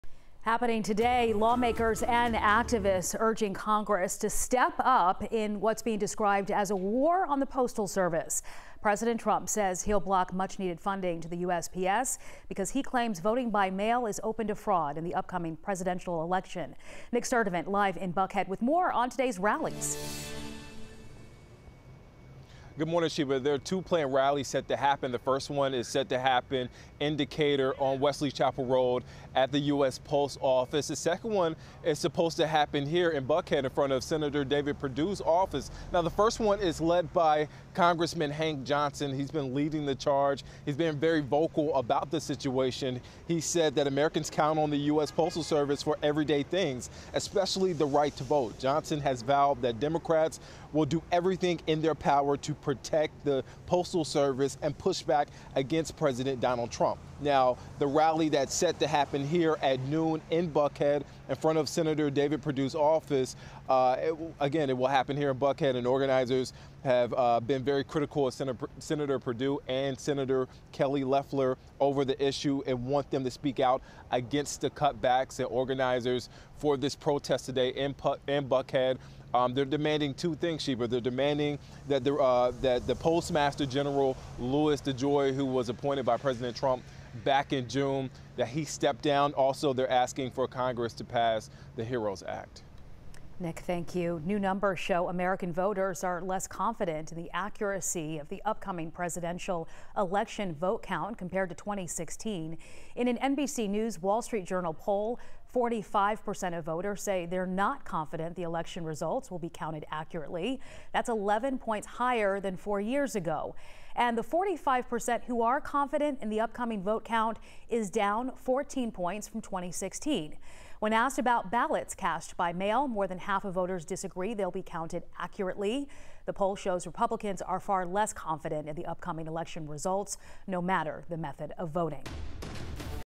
Two local post office rallies planned for metro Atlanta, voters less confidence in election results
The rallies will take place in Decatur and Buckhead.